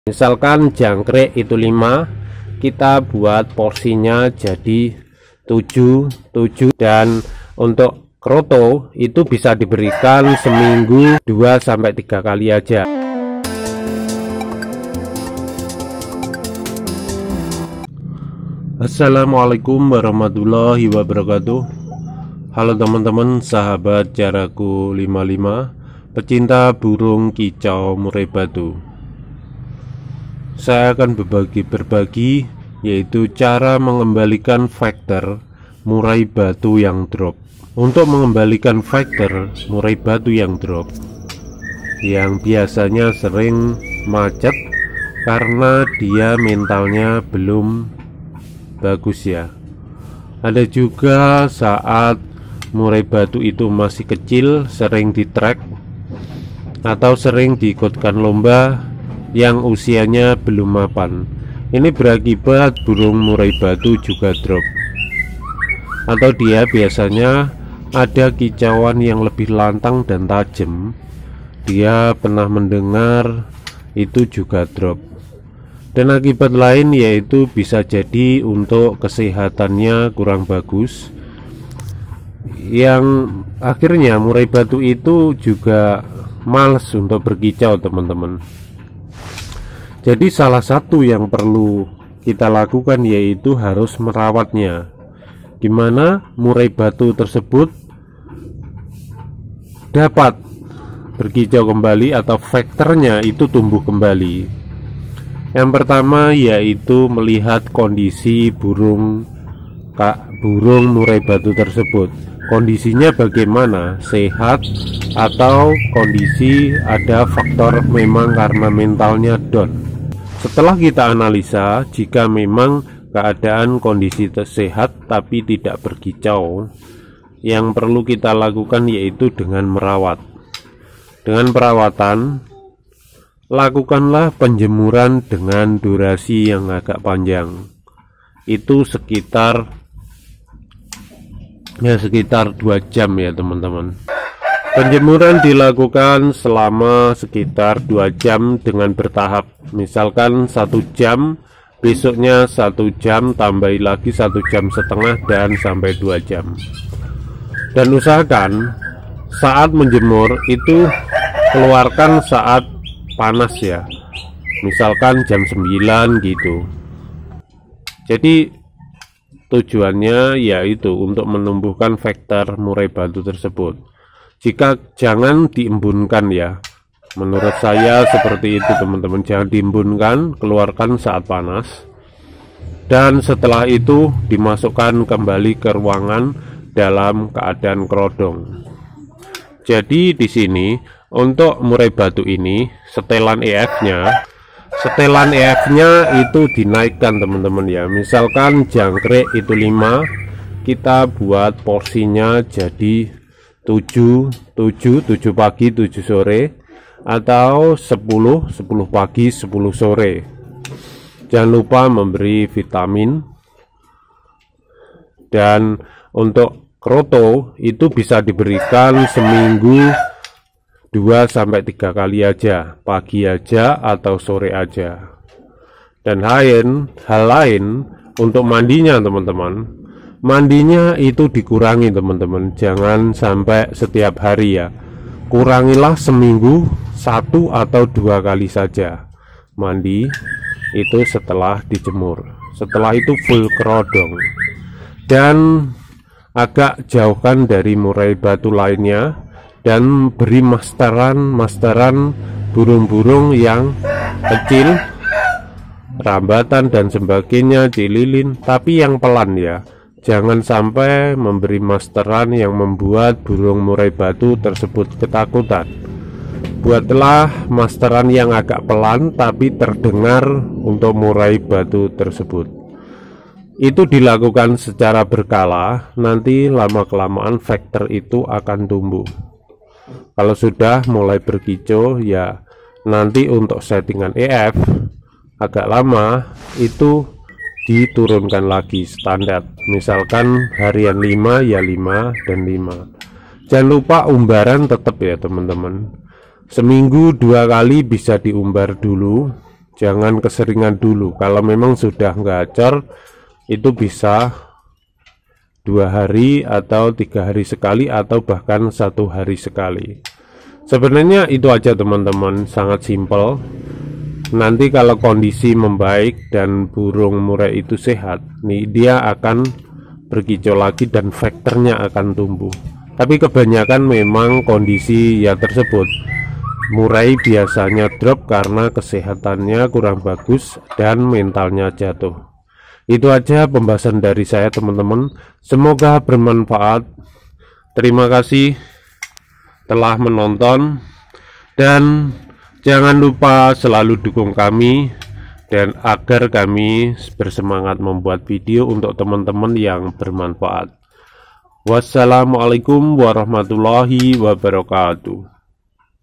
Cara Mengembalikan Fighter Burung Murai Batu Yang Drop
Murai batu fighter drop biasanya akan macet bunyi atau jarang berkicau sehingga sangat perlu di kembalikan lagi agar murai mau berkicau